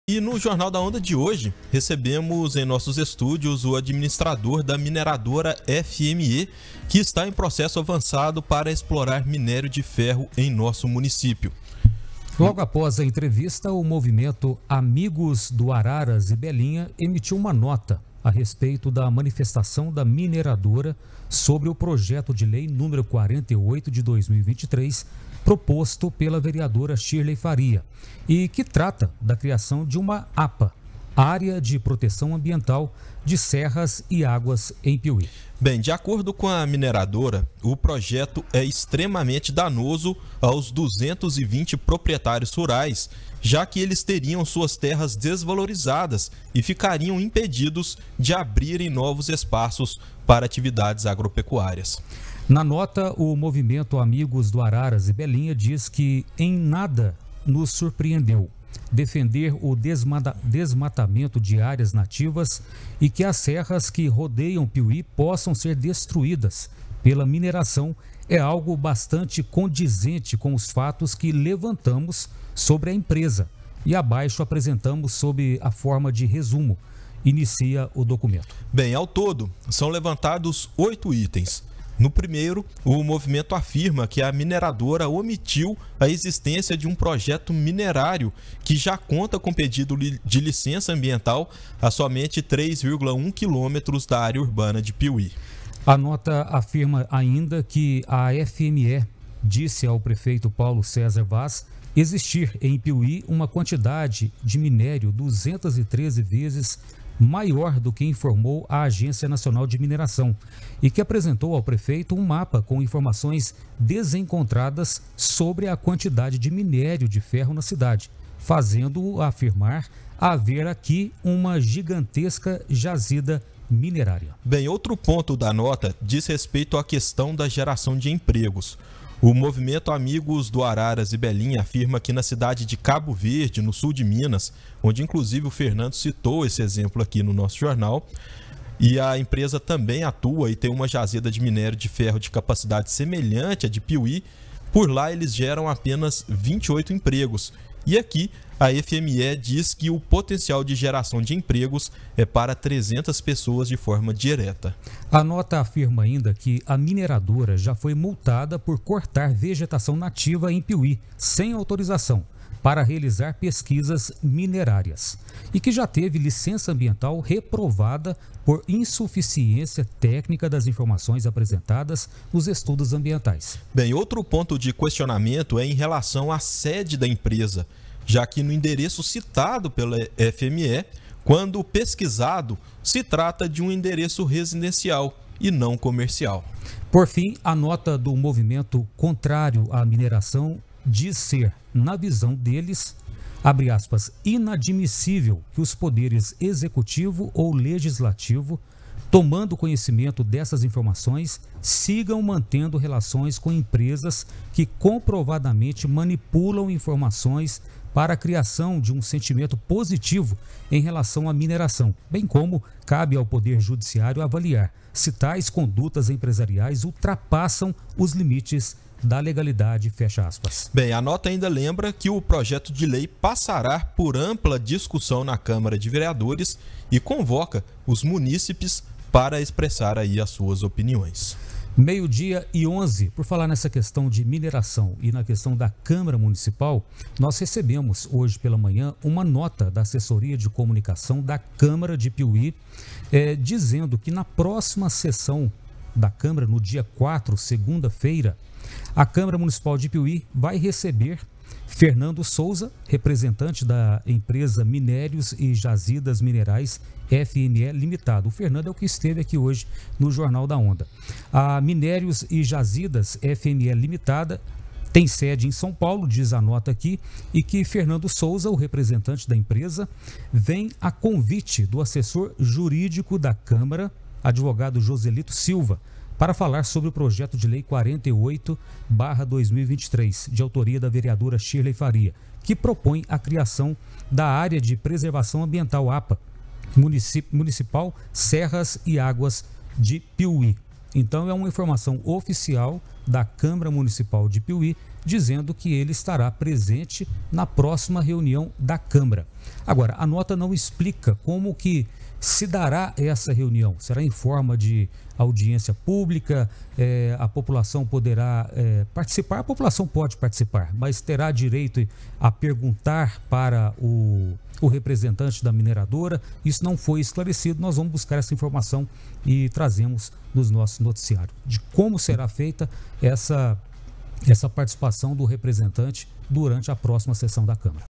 AMIGOS DO ARARAS E BELINHA EMITE NOTA APÓS ENTREVISTA DA ONDA OESTE COM REPRESENTANTE DE MINERADORA
| PIUMHI |

No Jornal da Onda de hoje recebemos em nossos estúdios o administrador da mineradora FME que está em processo avançado para explorar minério de ferro em nosso município.

Logo após a entrevista, o movimento “Amigos do Araras e Belinha” emitiu uma nota a respeito da manifestação da mineradora sobre o projeto de lei 48/2023, proposto pela vereadora Shirley Faria e que trata da criação de uma APA (área de proteção ambiental) de serras e águas em Piumhi.

Saiba mais: